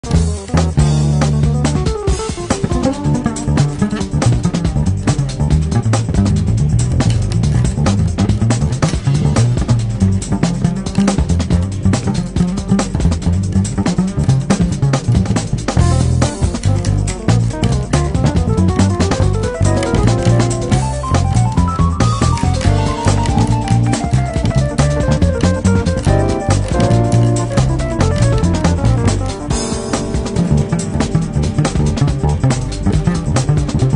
Elon Musk Plays Bass 🎸
Elon Musk ripping up the bass. I’m amazed he has time for this 😂